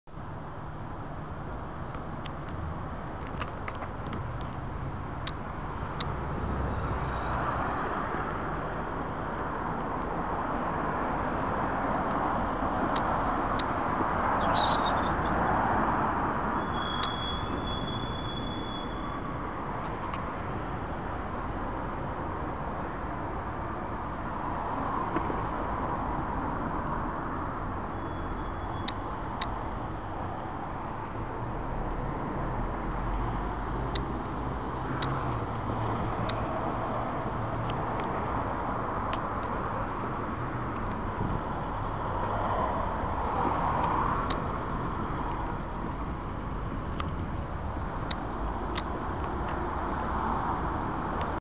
mercedes sl350 amg
this was parked outside an xtra-vision near st.stephens green